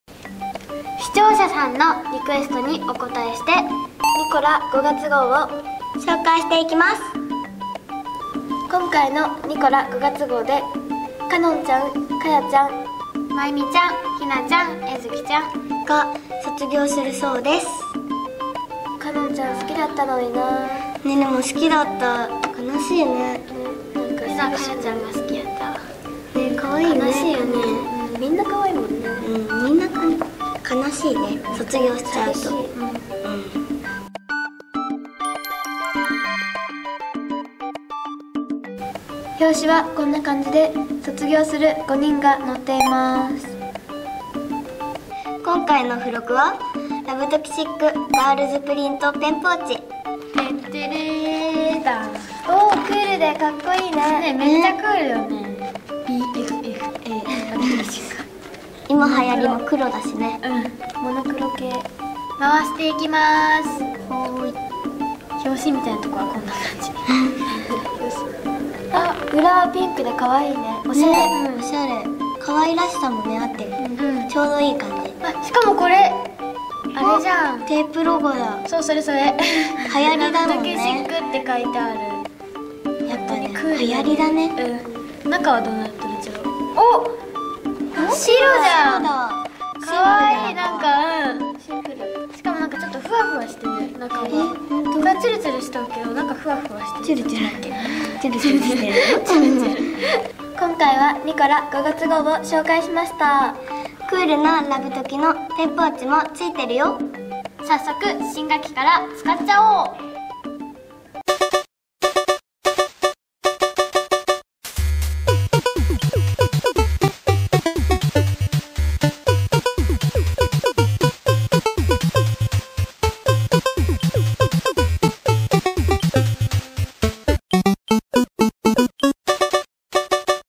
【ニコラ5月号】ラブトキのペンポーチが超COOL！「筆箱紹介」
((っ´ω`)ニコラ５月号にはなんと！ラブトキシックの筆箱が付いてくるよ！♥(´ω`⊂ ))